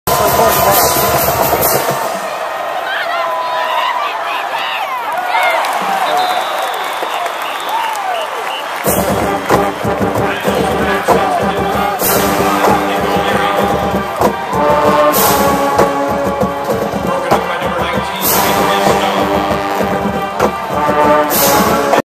NIU Defense Orange Bowl 2013
Northern Illinois University played Florida State University in the Discover Orange Bowl at Sun Life Stadium in Miami Gardens. The video includes Miami's Jamaal Bass (6), a starting linebacker for NIU and Miami's Demetrius Stone (19), a starting defensive back for NIU. Bass played for the East Miramar Wolverines, Miami Gardens Chargers and Miramar High School. Stone competed for the Golden Glades Panthers and American Senior High School.